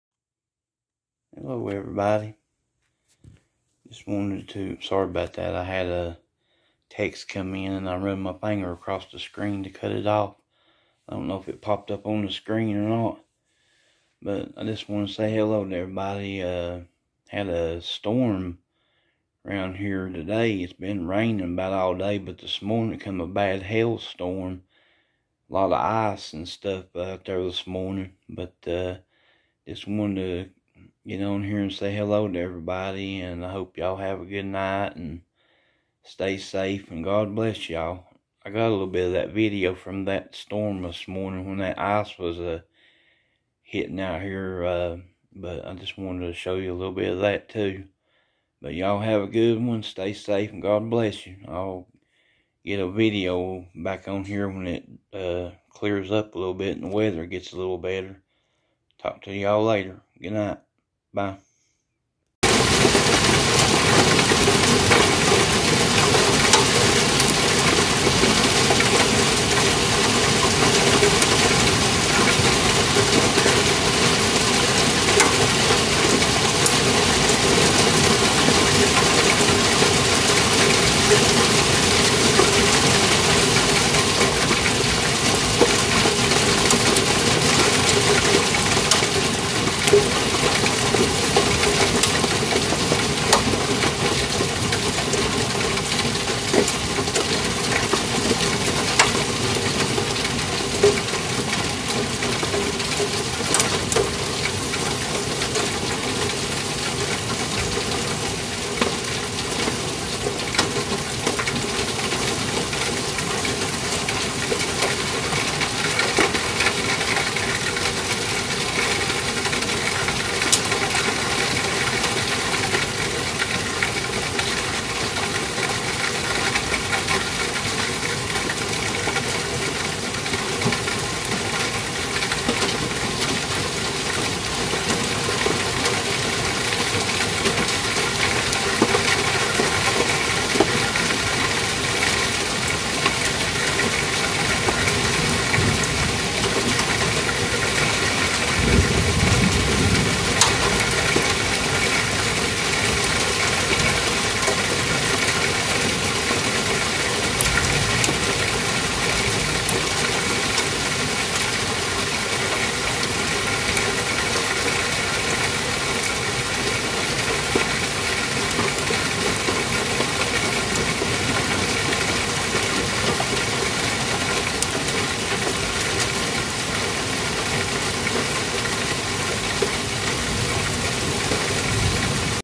Hello Hope Everyone Had A Good Day🙂 Got Rain About All Day And A Hail Storm This Morning 🌧️
03/28/2021
 Rained 🌧️ About All Day And A Hail Storm This Morning 🙏 We're Okay 🙂